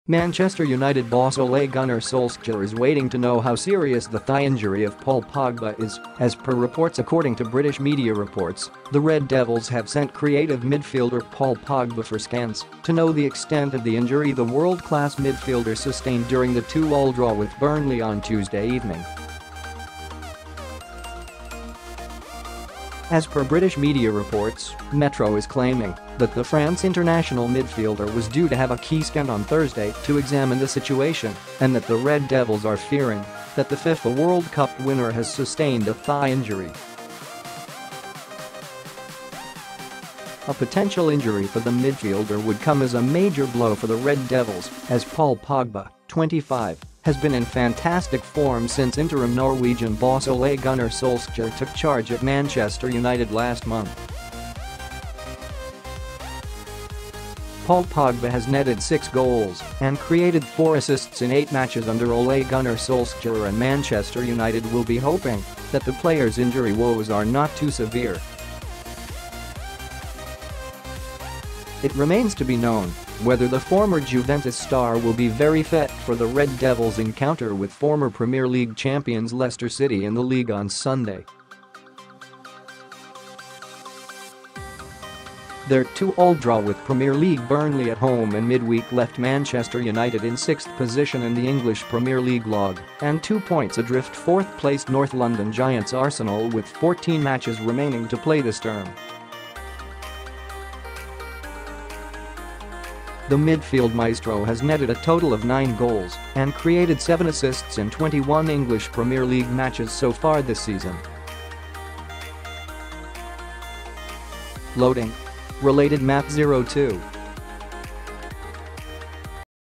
MASSIVE INJURY BLOW: Man United to be without world best midfielder for Leicester City clash on Sund
Manchester United boss Ole Gunnar Solskjaer is waiting to know how serious the thigh injury of Paul Pogba is, as per reports  According to British media reports, the Red Devils have sent creative midfielder Paul Pogba for scans to know the extent of the injury the world class midfielder sustained during the 2-all draw with Burnley on Tuesday evening.  As per British media reports, Metro is claiming that the France international midfielder was due to have a key scan on Thursday to examine the situation and that the Red Devils are “fearing” that the FIFA World Cup winner has sustained a thigh injury.  A potential injury for the midfielder would come as a major blow for the Red devils, as Paul Pogba, 25, has been in fantastic form since interim Norwegian boss Ole Gunnar Solskjaer took charge at Manchester United last month.   Paul Pogba has netted six goals and created four assists in eight matches under Ole Gunnar Solskjaer and Manchester United will be hoping that the player’s injury woes are not too severe.  It remains to be known whether the former Juventus star will be very fit for the Red Devil’s encounter with former Premier League champions Leicester City in the League on Sunday.  Their 2-all draw with Premier League Burnley at home in midweek left Manchester United in sixth position in the English Premier League log and two points adrift fourth placed north London giants Arsenal with 14 matches remaining to play this term.  The midfield maestro has netted a total of nine goals and created seven assists in 21 English Premier League matches so far this season.  Loading...  Related Matt 02